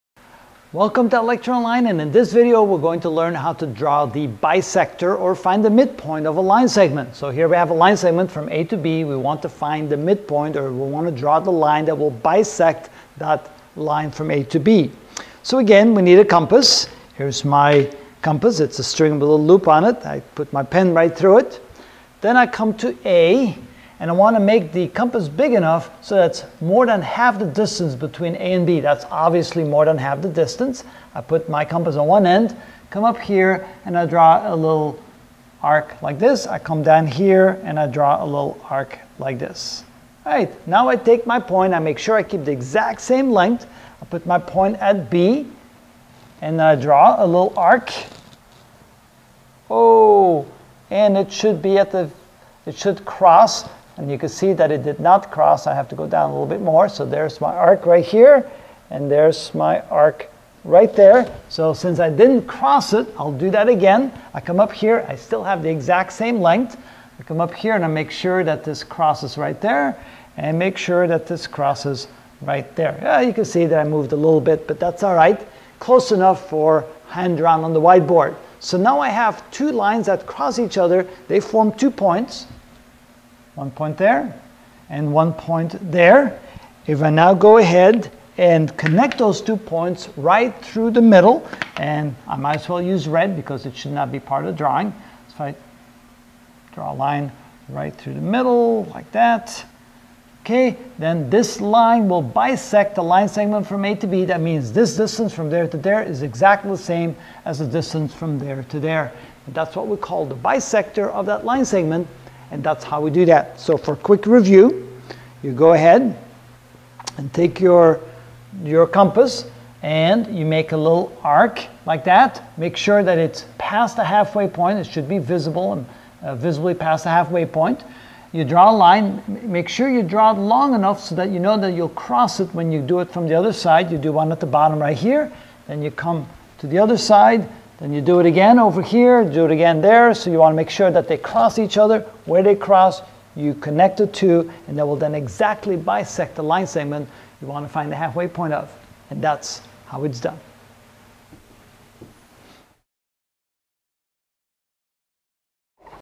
6.Geometry Constructions (6 of 15) How to Draw the Bisector of a Line Segment
How to Draw the Bisector of a Line Segment | By Predestined Future Global Leaders Academy